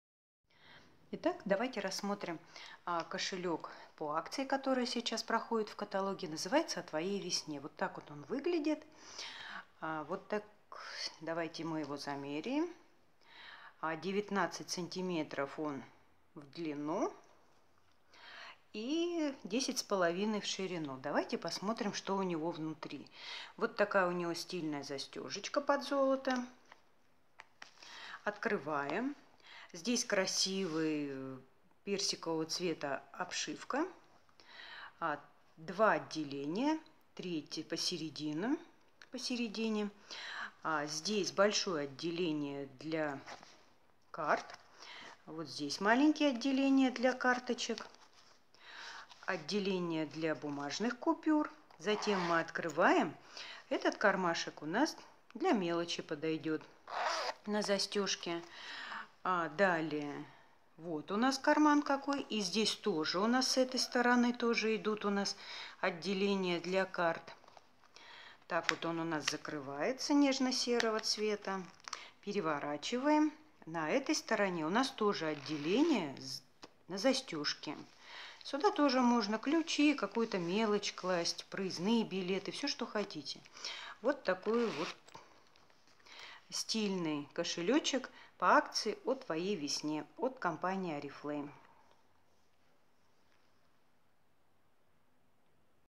Кошелек женский по акции О твоей весне. Показываю как кошелек выглядит внутри
В этом видео показываю как выглядит женский кошелек по Акции "О твоей весне", которая проходит с 28  января по 31 марта 2018 года.
Кошелек-код 29946

Кошелек- одна из важных деталей образа, которая должна быть универсальной и в то же время стильной. Кошелек изготовлен из экокожи,подойдет как на каждый день, так и для особого случая.
В кошельке есть отделения для карточек, для больших купюр. Для мелочи отдельно предусмотрены два кармашка на молнии: один внутри кошелька находится, другой- снаружи.

Нужна работа/подработка?
или хочешь покупать косметику со скидкой, напиши мне любым тебе удобным способом

Остались вопросы? Пиши,помогу, расскажу подробно!